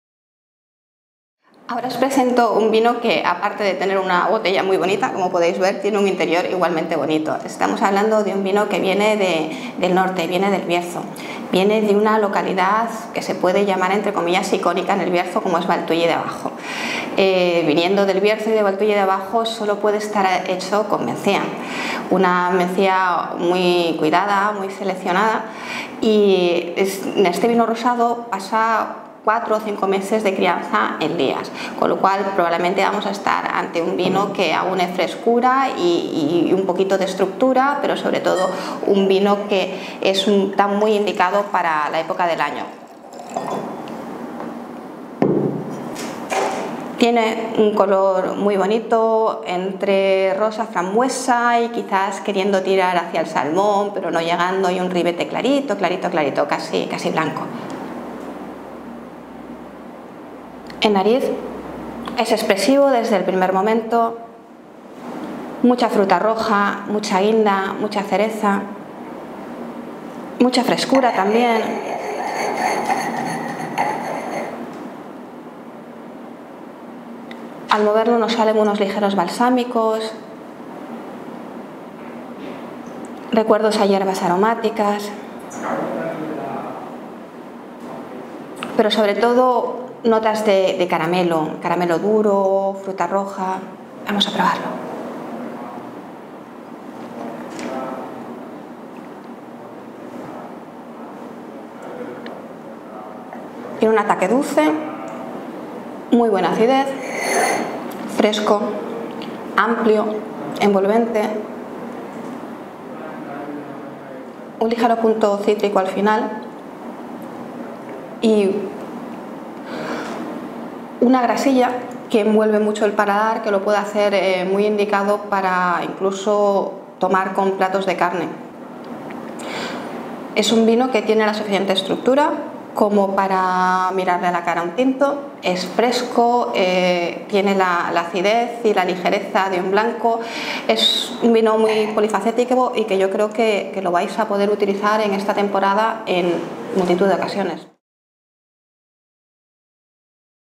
Cata del vino Peique Rosado 2014 - Familia de Vinos Peique
Peique Rosado 2014 forma parte del lote del mes de junio de 2015 del Club Verema

Peique Rosado 2014 es un rosado venido del Bierzo profundo y elaborado cómo no con Mencía, la reina del Bierzo. Un vino con una grandísima estructura, gracias a su crianza de 4 o 5 meses sobre lías en depósito inox. Frutal, goloso y cítrico.

Peique Rosado 2014 está valorado con 93 puntos sobre 100 por los usuarios de Verema

Más información del lote del Club Verema de junio de 2015